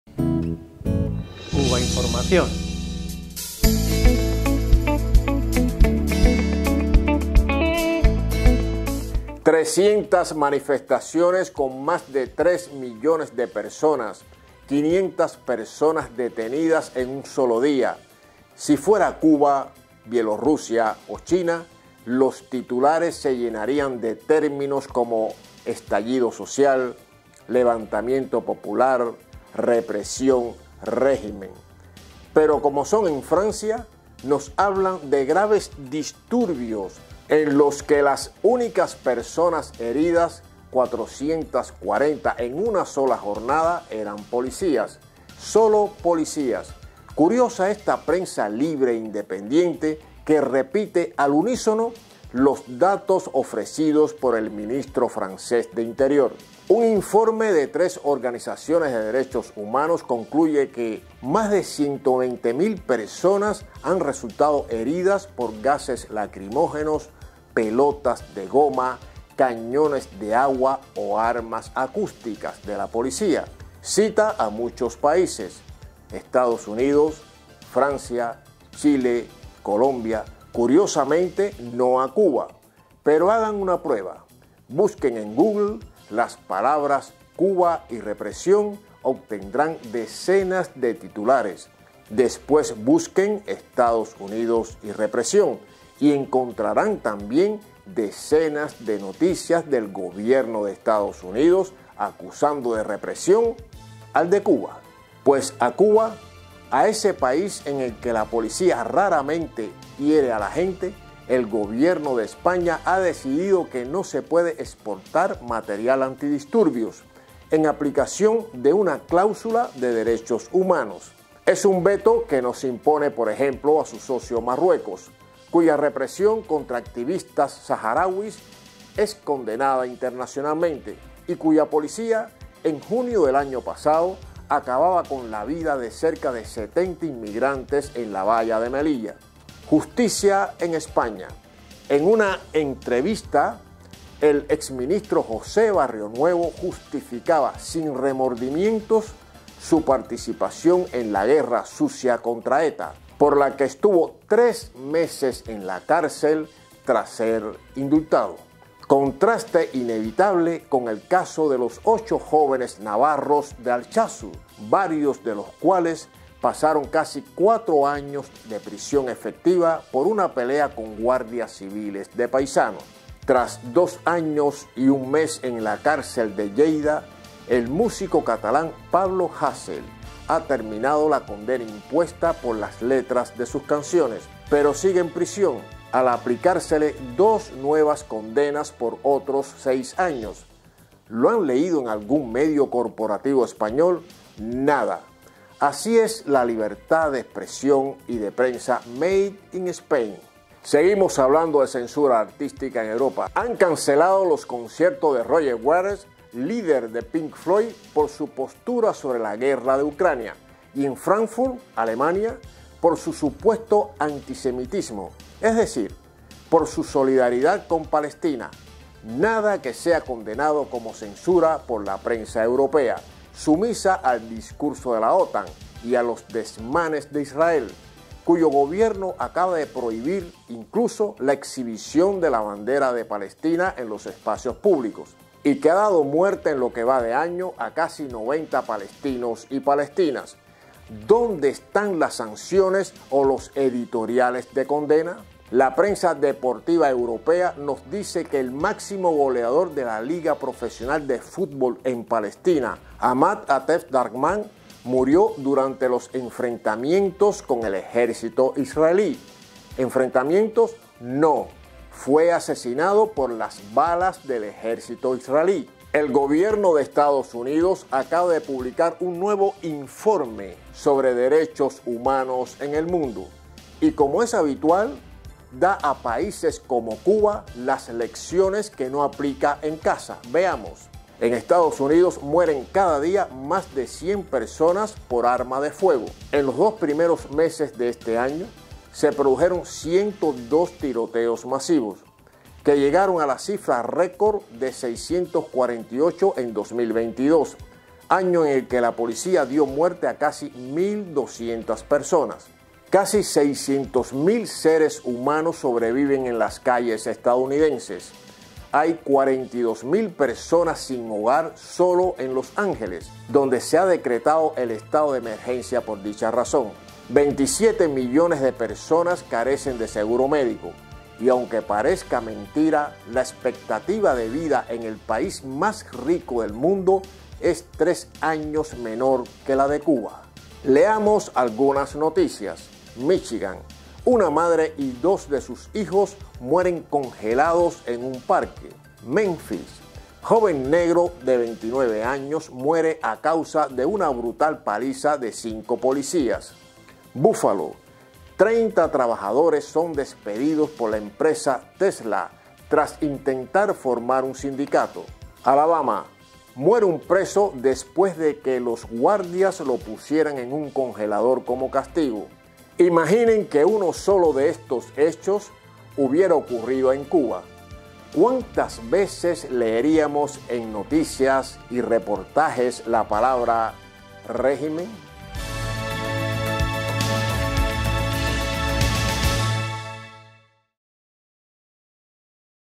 ¿Si las protestas en Francia fueran... en Cuba?
300 manifestaciones con más de tres millones de personas, 500 personas detenidas en un solo día. Si fuera Cuba, Bielorrusia o China… los titulares se llenarían de términos como “estallido social”, "levantamiento popular", "represión", “régimen”…
Fuente original en Cubainformación TV: